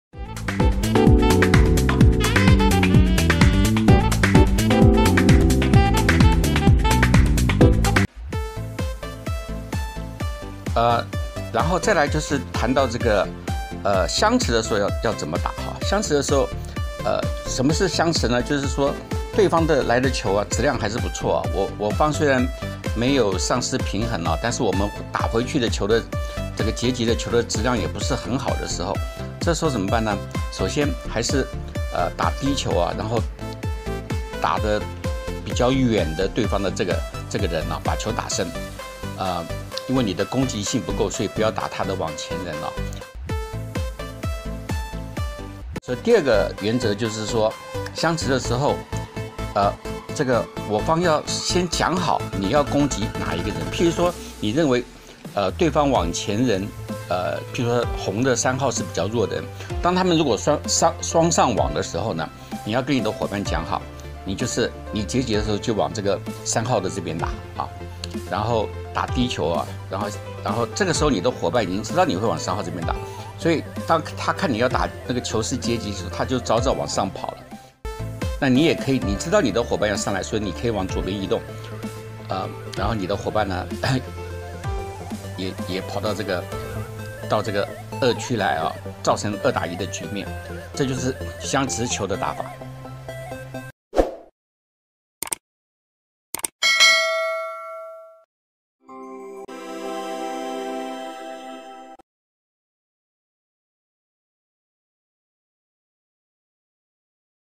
网球 - 双打战术 -- (九) 双打在相持战时, 如何运用打法夺点
练了网球技术, 同时练练战术. 中级程度球员双打时战术, 球打低打深, 集中打对手较弱球员